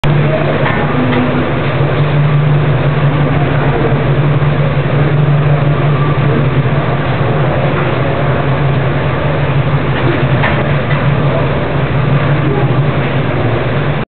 Wasserspiel (Water Park) Spitting Man
A quick clip of a spitting face at the Wasserspiel in Germany.